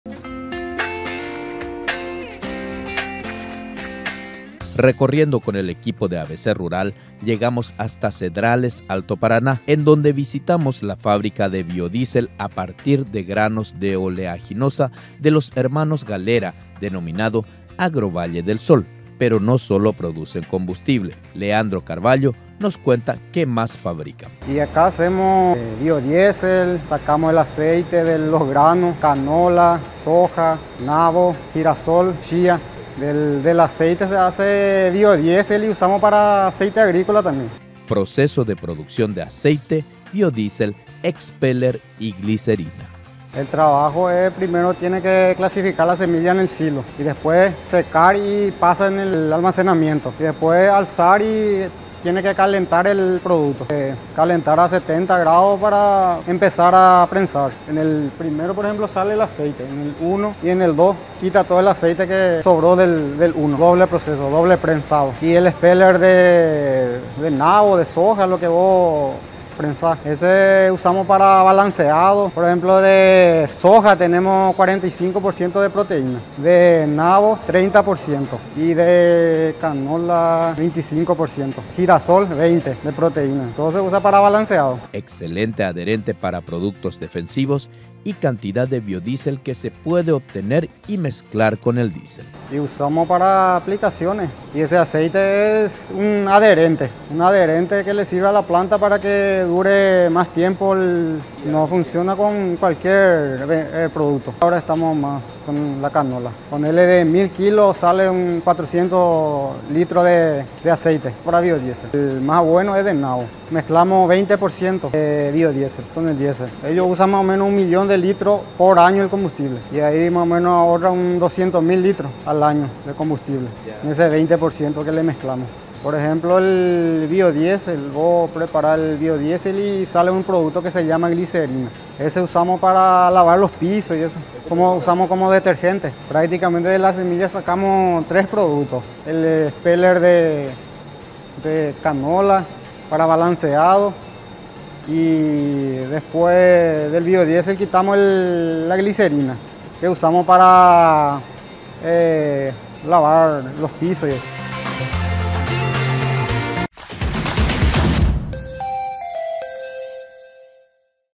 Fabrica de biodiesel, expeller, coadyuvante y glicerina
Recorriendo con el equipo de ABC Rural, llegamos hasta Cedrales, Alto Paraná, en donde visitamos la fábrica de biodiesel a partir de granos de oleaginosa de los Hermanos Galhera denominado Agrovalle del Sol. Pero no solo producen combustible, Leandro Carvallo nos cuenta que más fabrican.